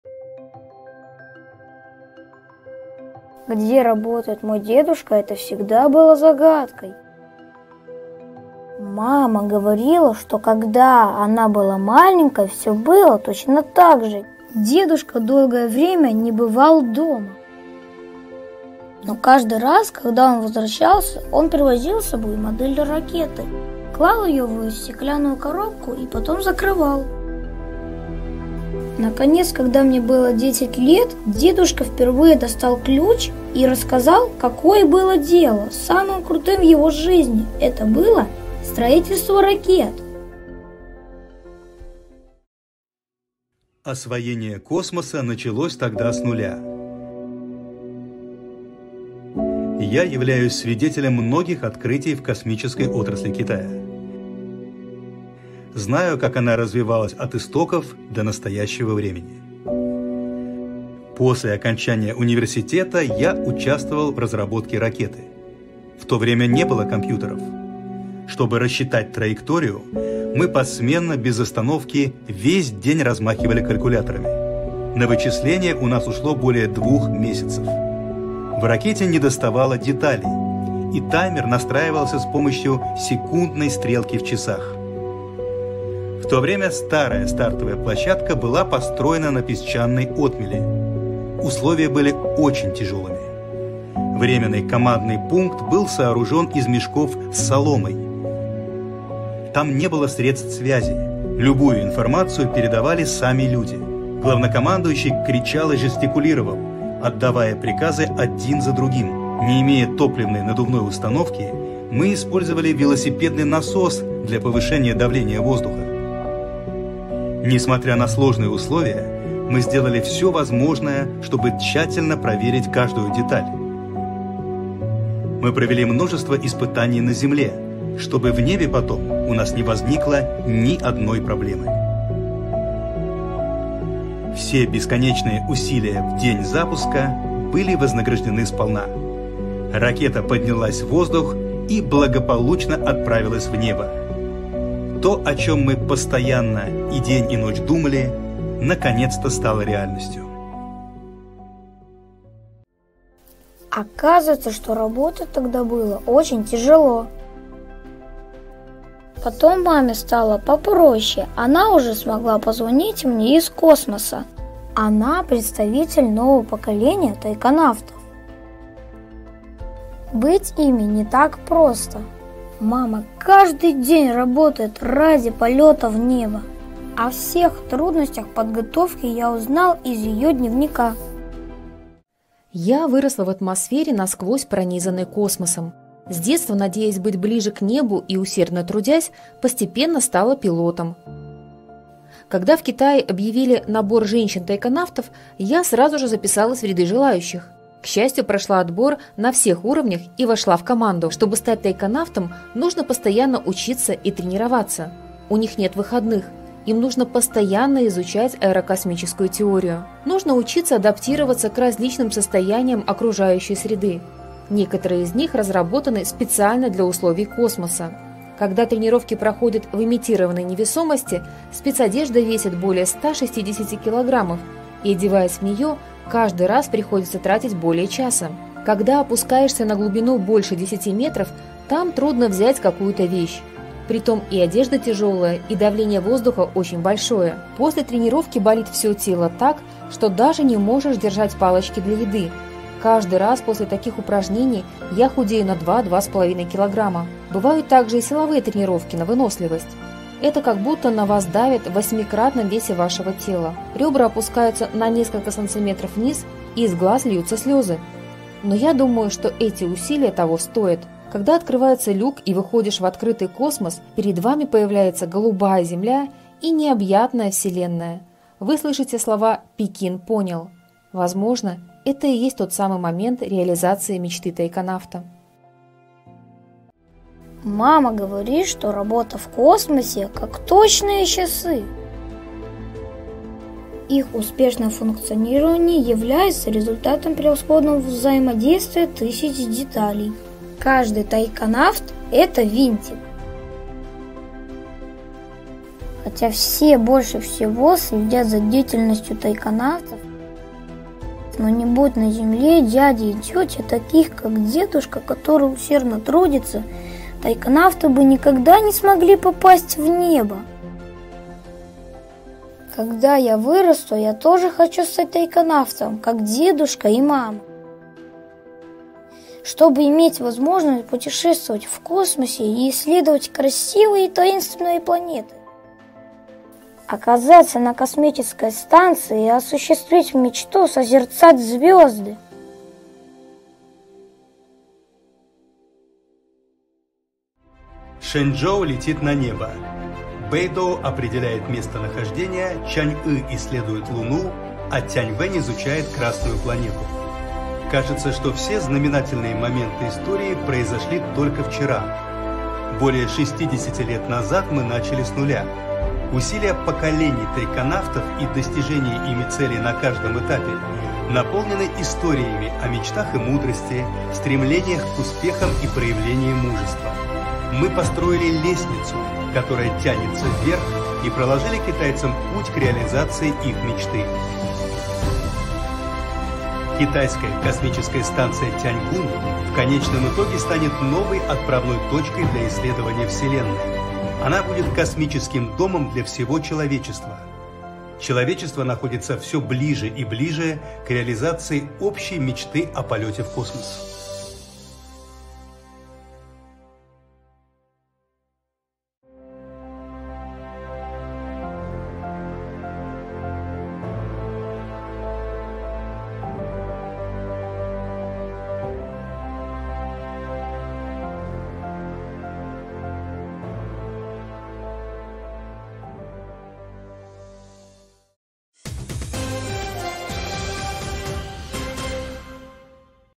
Расстояние до мечты: Путь Китая к освоению космоса
Более 60 лет назад Китай запустил свою первую зондирующую ракету «Т-7M». После долгих лет напряженных исследований и работы нескольких поколений космонавтов, КНР шаг за шагом продвинулась в направлении дальнего космоса.
Именно вера, ответственность и преемственность помогли воплощению китайской космической мечты.